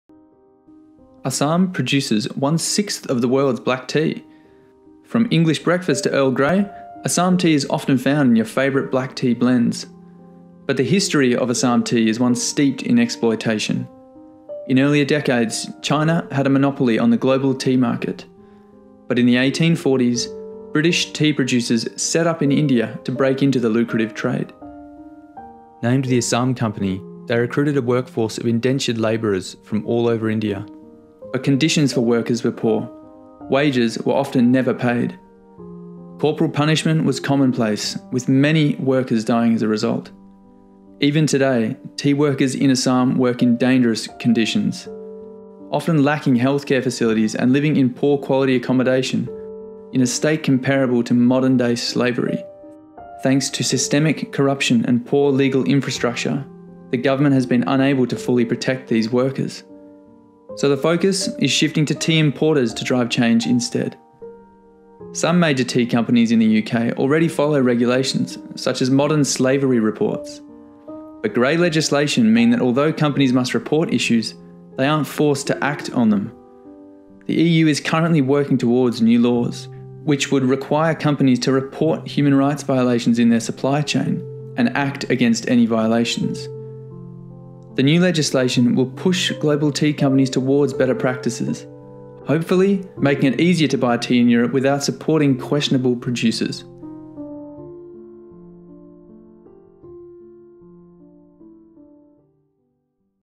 The True Cost Of Assam Black Tea
Have you ever wondered where your black tea comes from? Learn more about the dark social undercurrents that laid the foundation for one of the modern worlds largest tea producers

Motion Graphics by Jamie Burton
Script &. Narration by Oliver Fredriksson
Based on the article by Madhura Rao